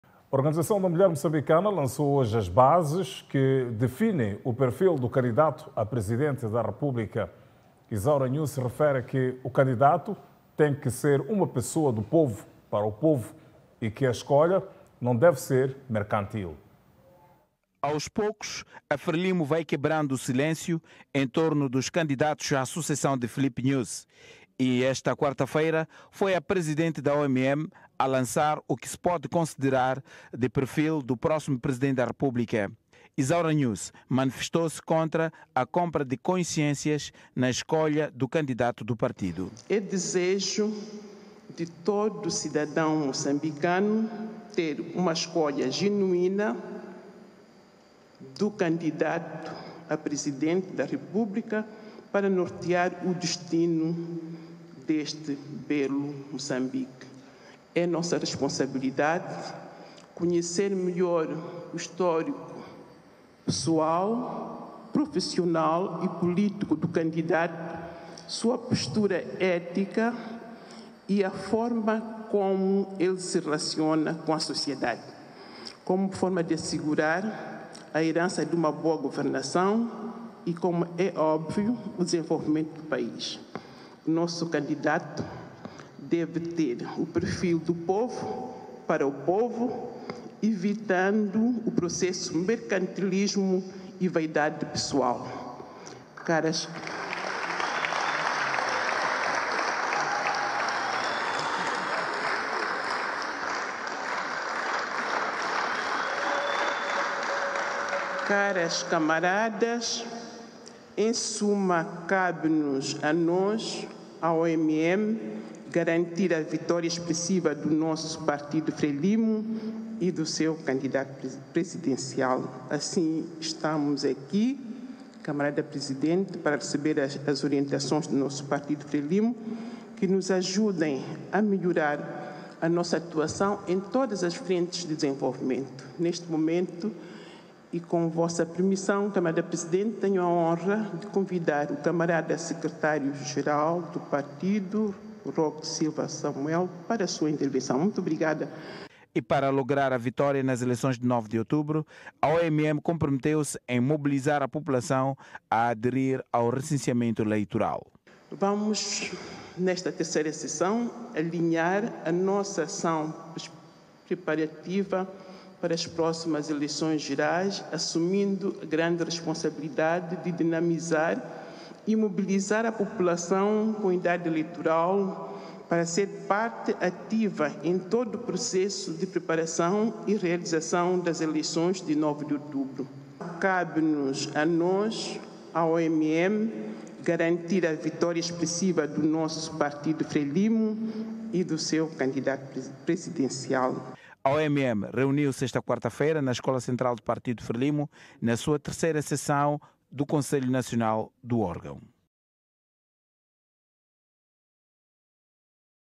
OMM QUER QUE O SUCESSOR DE NYUSI SEJA ALGUÉM COM HISTÓRICO E QUE TENHA BOAS RELAÇÕES COM A SOCIEDADE
Enquanto continua o "mistério" sobre quem é realmente a figura que vai suceder o actual presidente, Filipe Nyusi, a Organização da Mulher Moçambicana trouxe à superfície aquilo que é o seu intento sobre o perfil do próximo chefe do estado moçambicano "tem que ter histórico pessoal, profissional, político, com uma postura ética e boa relação com a sociedade", disse.

Isaura Nyusi e a Organização que preside, defendem que o sucessor de Nyusi com essas qualidades, deve também ser alguém que vai segurar uma boa governação e desenvolvimento do país.

Na ocasião, a OMM reitou o seu compromisso em trabalhar na mobilização da população para garantir a vitória da Frelimo nas eleições que se avizinham.